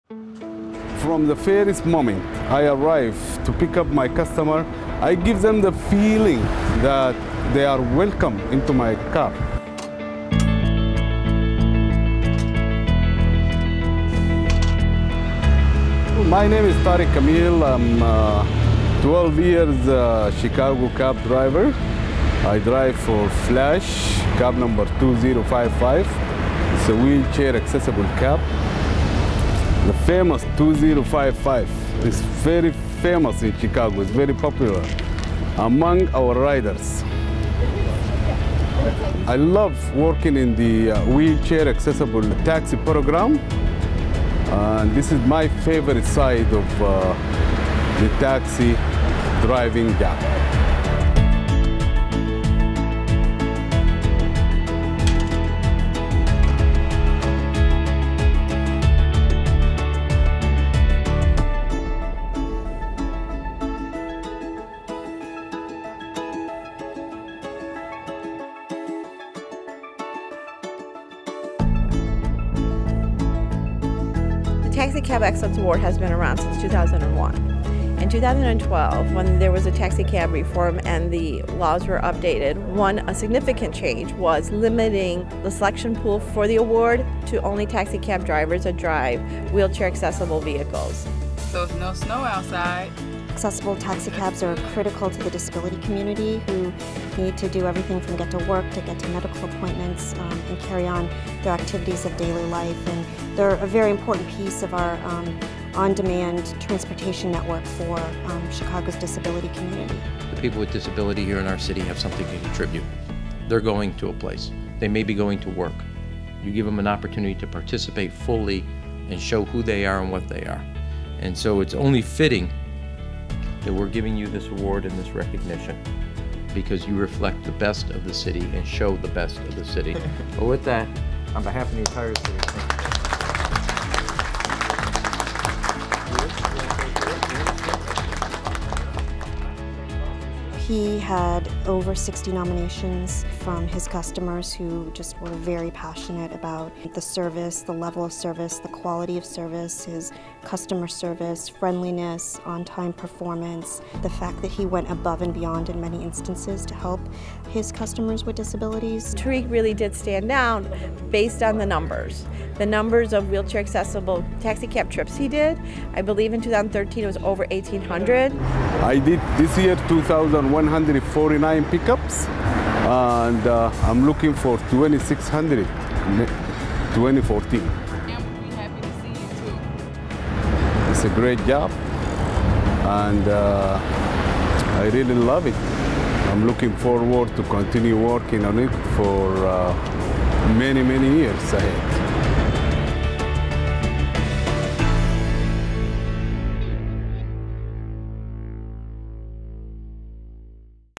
2013 Taxicab Driver Excellence Award
Mayor Emanuel recently presented the 2013 Taxicab Driver Excellence Award to longtime Chicago taxicab driver Tarig Kamill based on his outstanding service to the disabled community.

As part of the Mayor's taxi reform ordinance passed in January of 2012, a free medallion is awarded to taxicab drivers who provide exemplary service to the disabled community by driving wheelchair accessible vehicles (WAV). The average price of a Chicago taxi medallion is approximately $350,000.